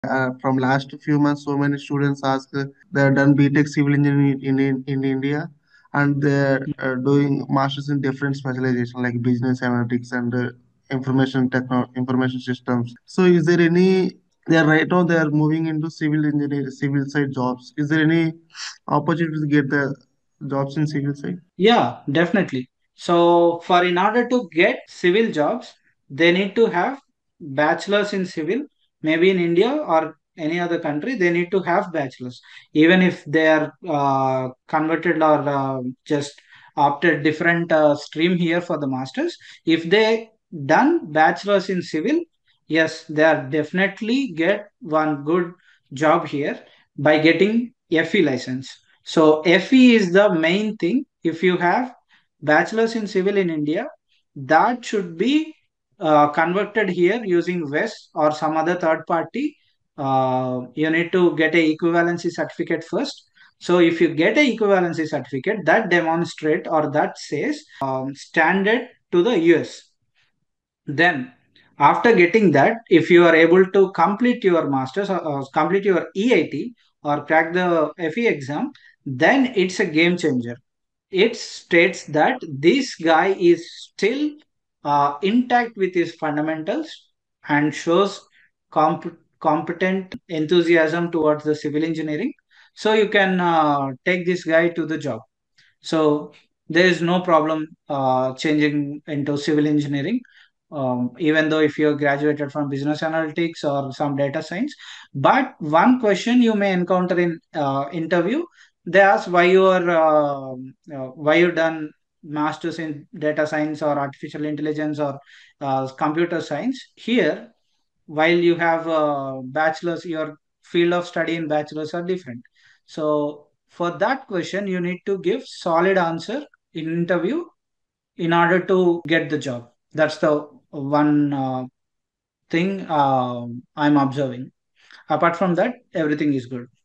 Did B.Tech Civil in India and Masters in another field – Can they get a civil job in the USA?
If you did B.Tech in Civil Engineering from India but your Master’s is in another field, can you still get a Civil Engineering job in the USA?  How your Bachelor’s degree, FE and PE exams matter, and what options you have to enter the civil engineering field in the USA even if your Master’s is different. Watch till the end to plan your career wisely.